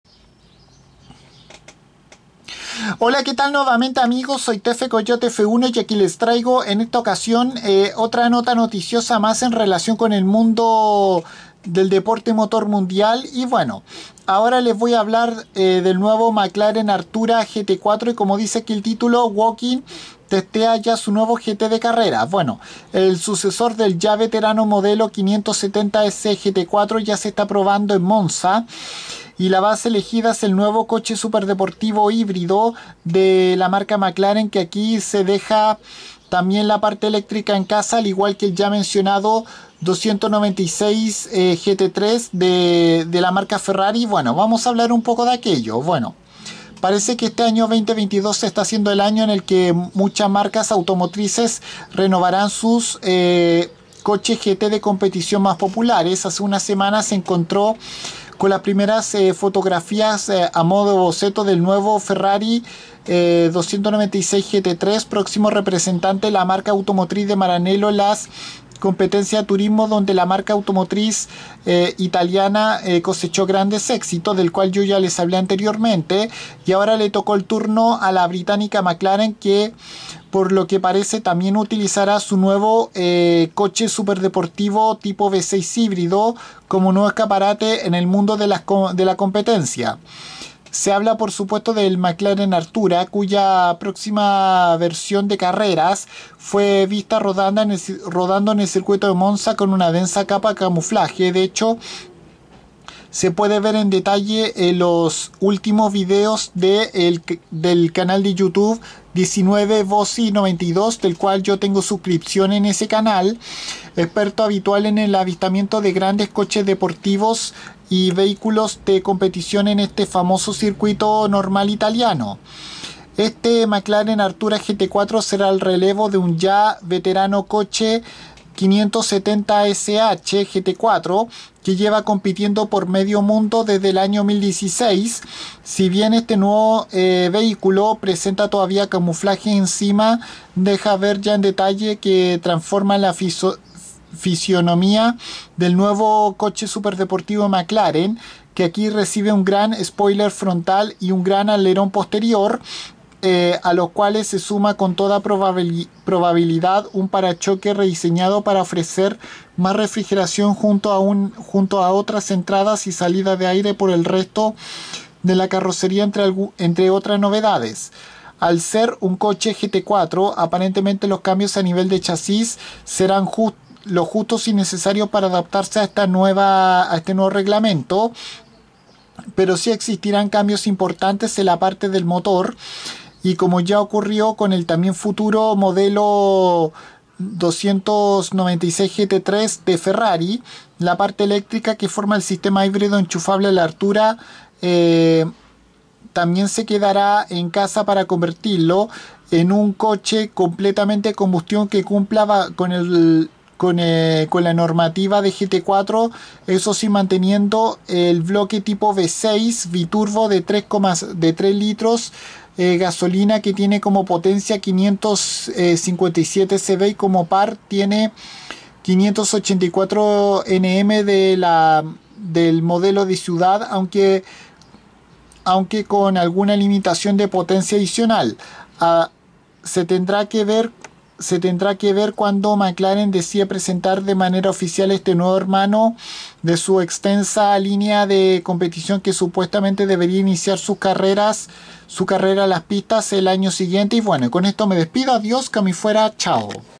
Mclaren Artura GT4 información
En  este vídeo les explico sobre como es esta versión de carreras del nuevo coche superdeportivo híbrido enchufable de la marca automotriz de Woking que al igual que su futuro rival el Ferrari 296 GT3 también dejará de lado su parte eléctrica y que será el nuevo sucesor del ya veterano modelo 570S GT4